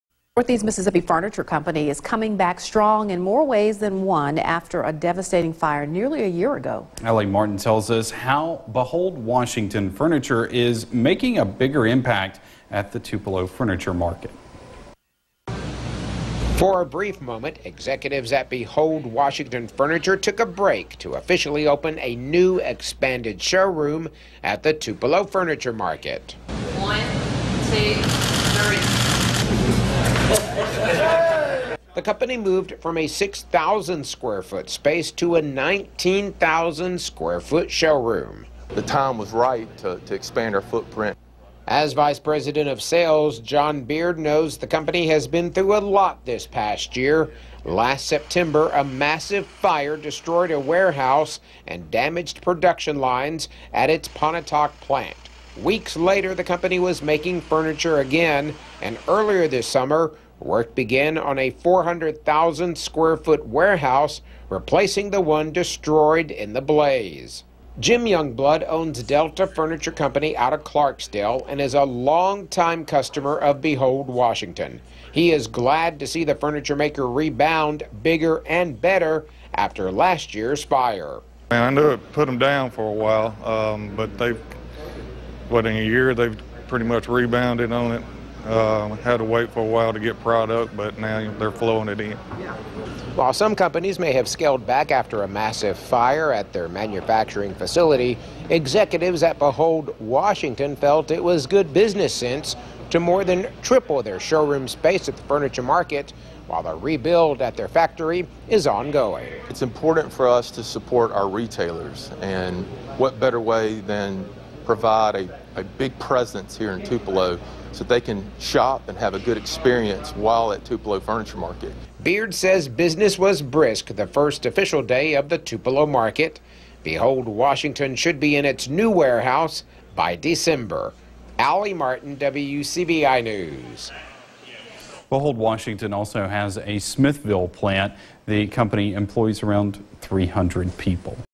Market Renew
A northeast Mississippi furniture company is coming back strong in more ways than one after a devastating fire nearly a year ago.